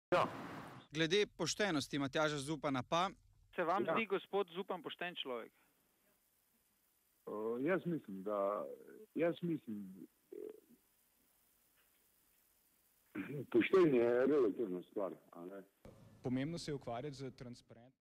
Ali je Matjaž Zupan pošten? Odgovor direktorja Stroke: "Poštenje je relativna stvar!"
Novinar POP TV Jure Bračko je v prispevku objavljenem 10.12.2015 na POP TV direktorja stroke Sandija Markona vprašal: "Se vam zdi gospod Zupan pošten človek?" Po dolgem premoru in jecljanju je sledil ogovor "Poštenje je relativna stvar".